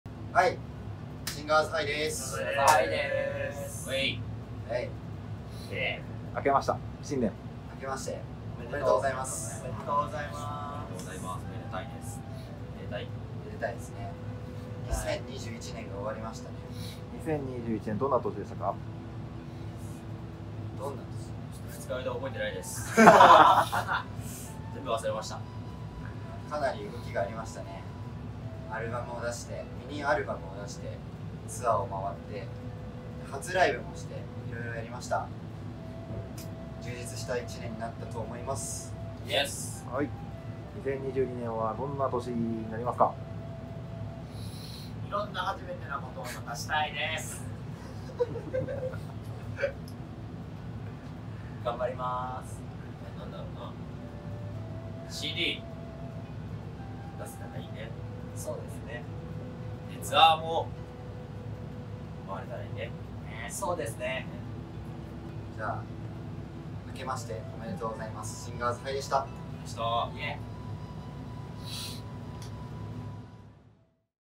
【シンガーズハイ】新年のごあいさつ2022
新年明けましておめでとうございます！シンガーズハイから新年のごあいさつです！

▼各アーティストダイジェストはこちら